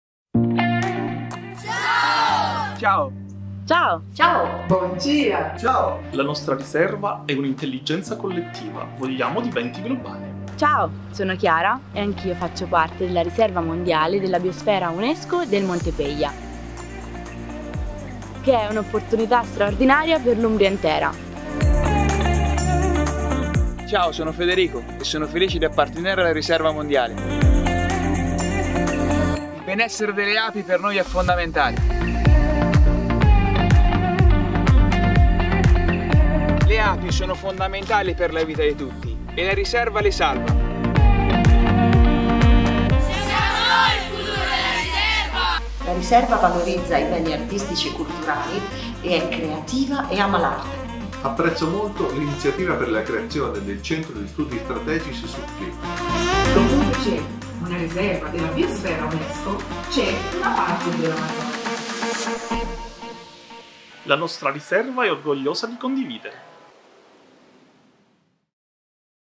Riserva Mondiale della Biosfera Unesco del Monte Peglia - Umbria -Italia
La Riserva della Biosfera Unesco del Monte Peglia ha ottenuto nel 2018 il titolo Mab Unesco , il riconoscimento internazionale per la biodiversità.

La Riserva si trova in un’ampia zona vulcanologica e forestata, al centro dell’Umbria e quindi dell’Italia, ponte e punto di congiunzione tra le Riserve naturali d’Italia e d’Europa, dalle Dolomiti e dalla Mitteleuropa all’Etna, dal piccolo Vulcano, il nostro, al grande Vulcano e al Mediterraneo, e ai tempi delle crisi e delle emergenze climatiche, è messaggio simbolico fortissimo e travalicante.

La nostra Riserva è una intelligenza collettiva che vuole diventare globale.